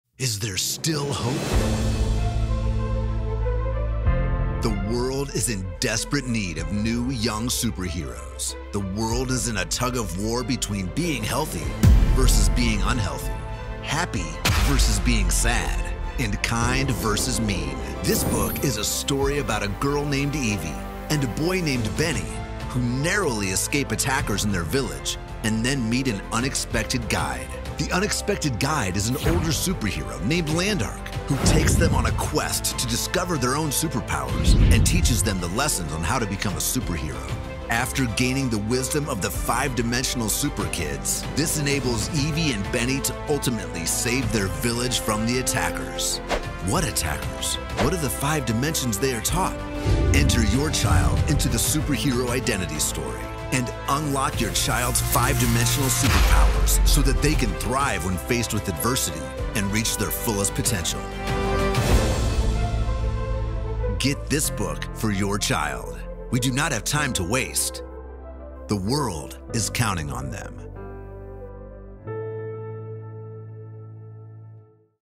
The Superhero Identity Book
Unlock your child's 5 Dimensional superpowers so that they can thrive when faced with adversity and reach their fullest potential..